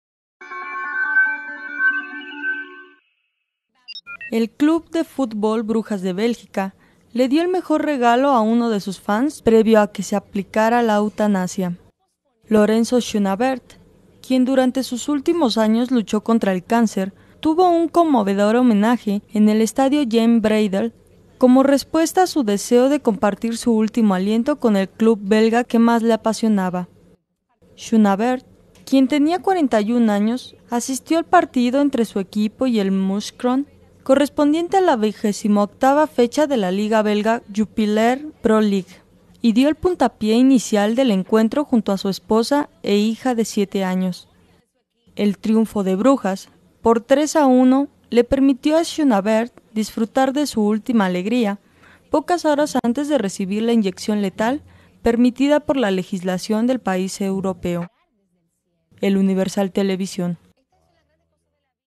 Club belga despide a su más grande fan antes de eutanasia
Lorenzo Schonbaert tuvo un conmovedor homenaje en el estadio Jan Breydel antes de morir